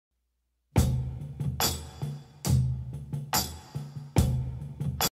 Realistic Walking man without flash once animation effect
Realistic walking man  video is made with out the help of flash once animation effect.